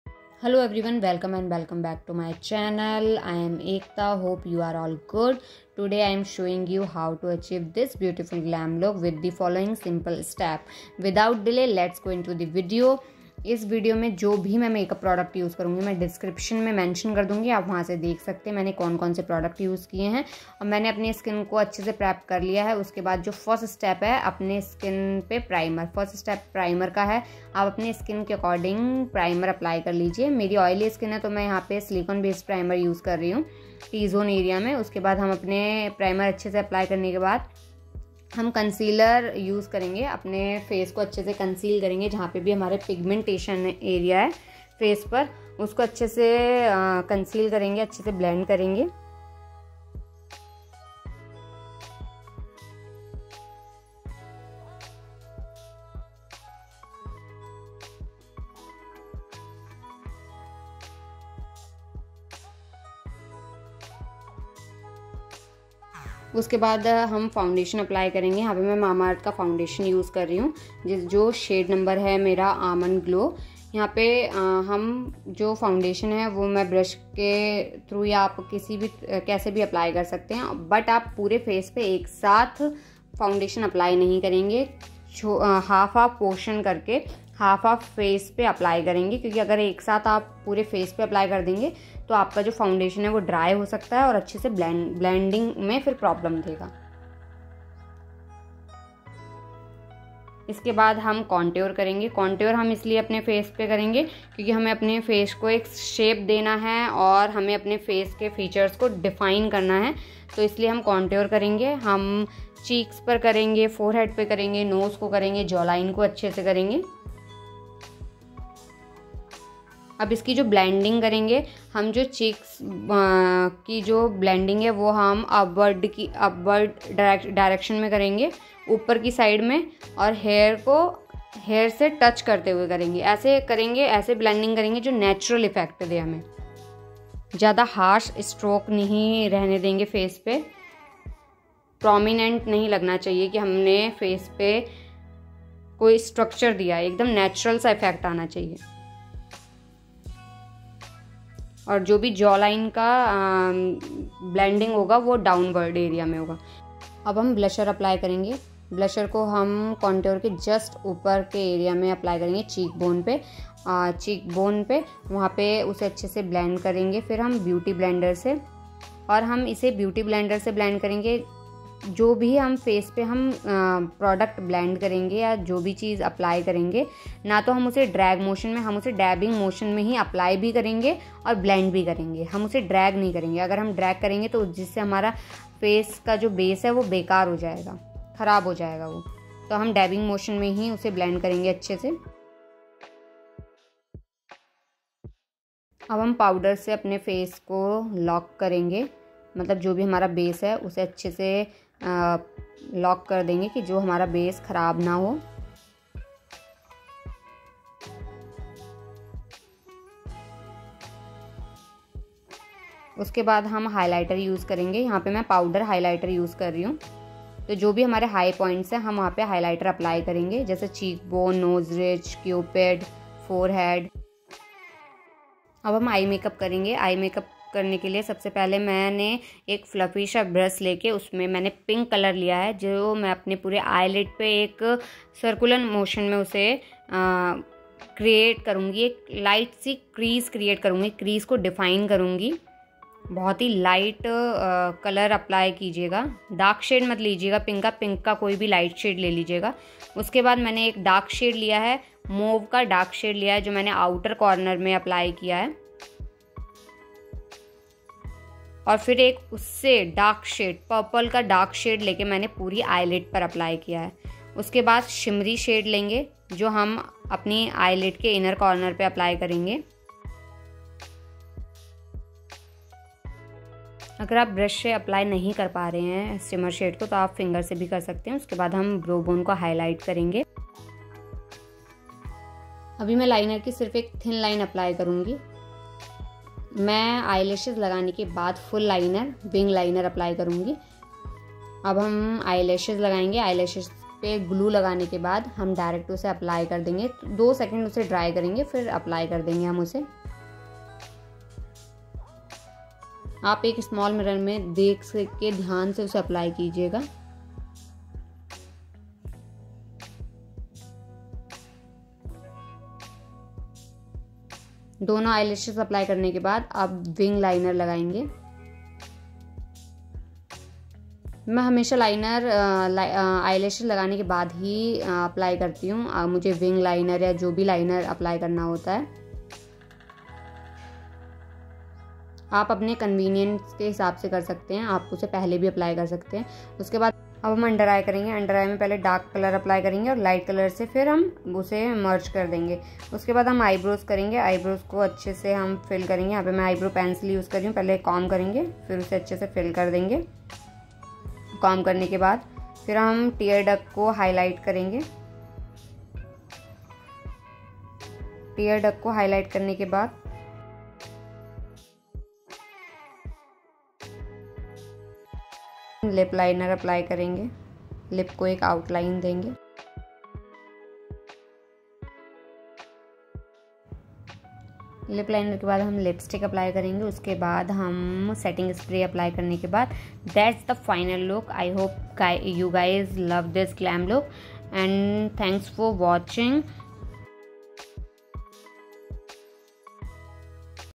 Glam look tutorial ✨️ | Ekta Bhadauria
products used:
Face :
1. Primer: base glory pore minimizing primer

3. Foundation: mamaearth serum

4. Contour: sugar cosmetic

5. Translucent powder: kay beauty

Eyes:
7.Eyeshadow pallete: makeup revolution

Lipstick:
10.Lipstick pallete:Mars

11. Makeup fixer: swiss beauty

yayy! you reached the bottom of the description box. comment "❤️"